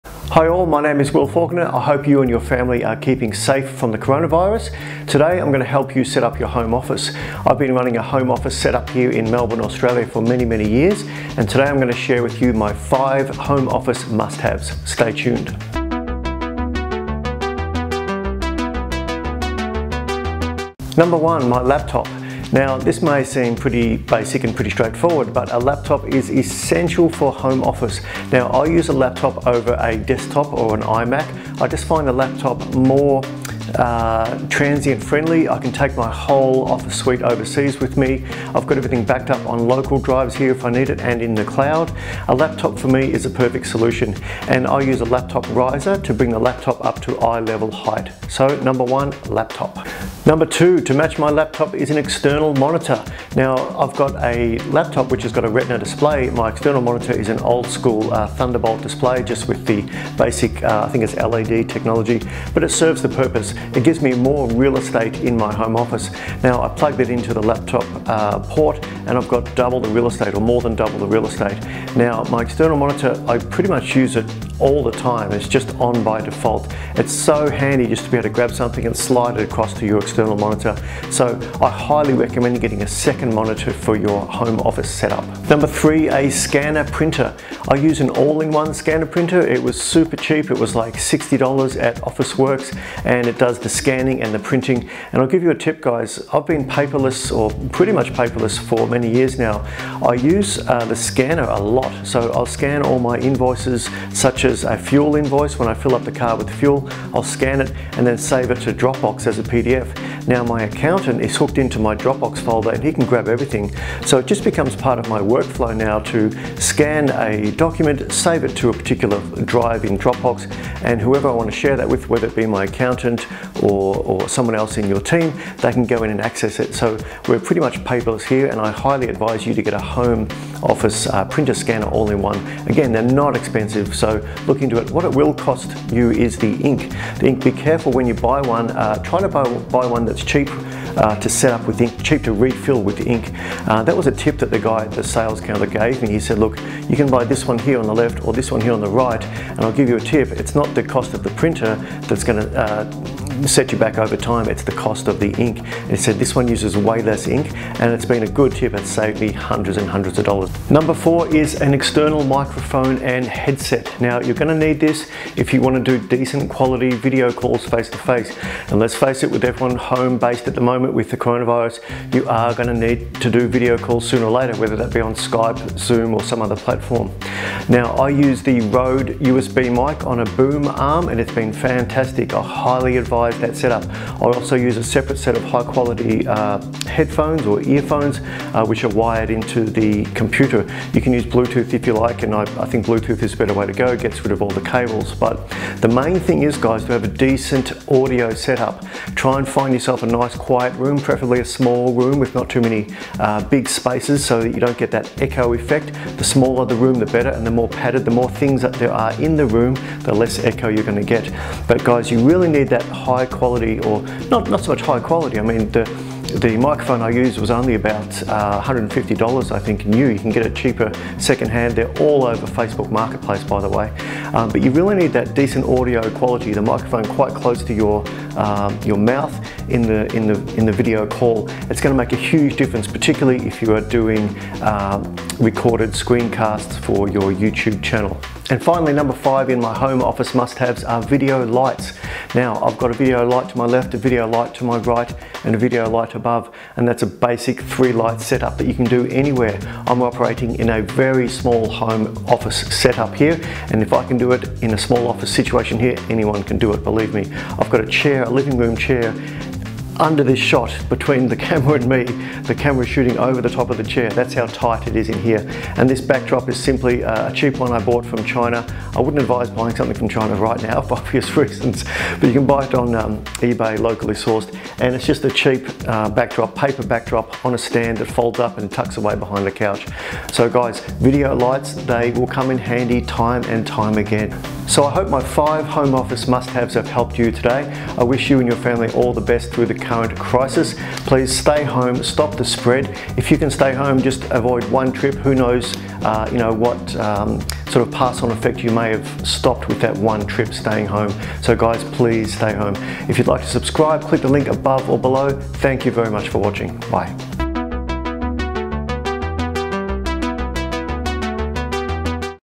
My 5 Home office must haves
In this video I share with you 5 items that I use every day in my home office. Hopefully it will help you.